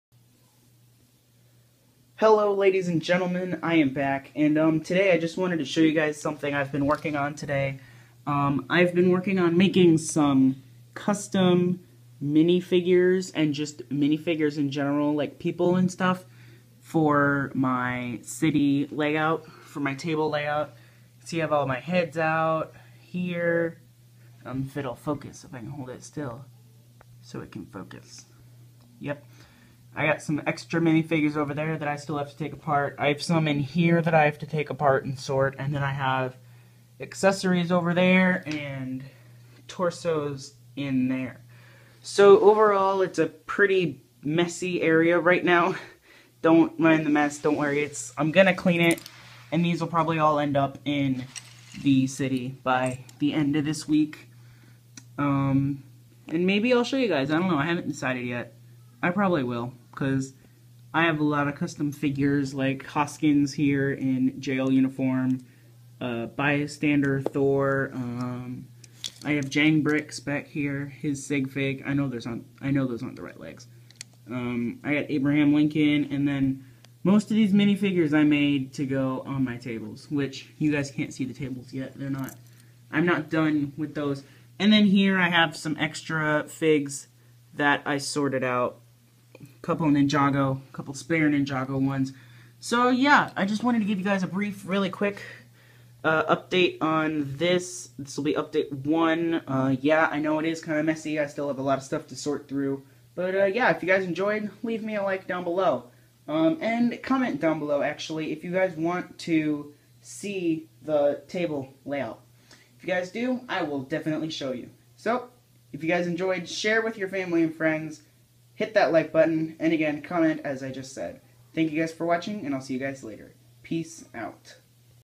Custom Minifigs for my Lego layout!!
Hey guys! I just wanted to give a brief overview of what I have been working on for the layout!!!